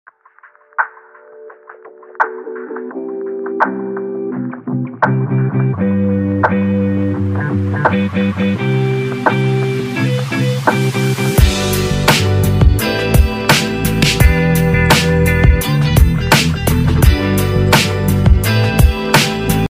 Seattle DJ goes to Boise for a wedding! BanBury Golf Course in Eagle, ID
Beautiful day for an indoor/outdoor reception in Boise. Yeah, we like to travel 😄